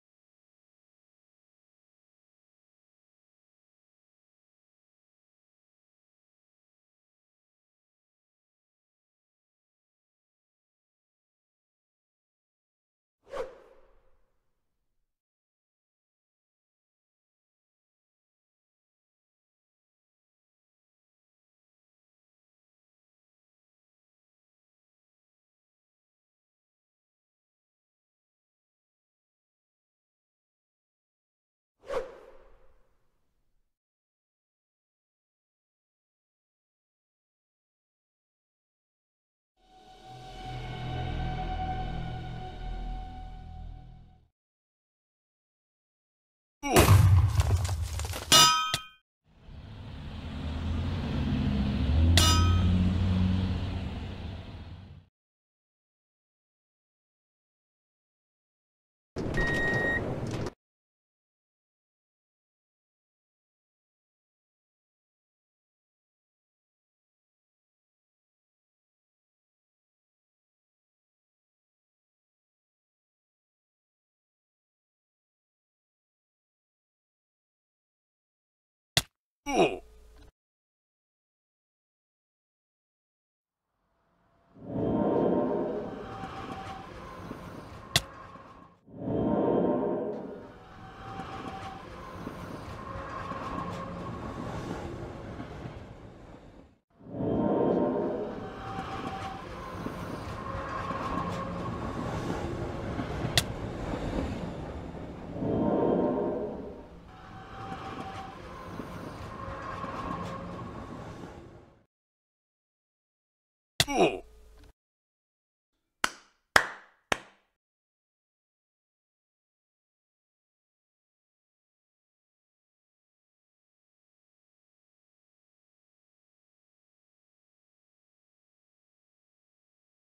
Agam's extremely unlucky day
I encourage development for the Rowville Rail - whether it be heavy or light rail!
In this video of Agam, he is punished by (buddha with) some very unusual things.
Created with Mine-imator.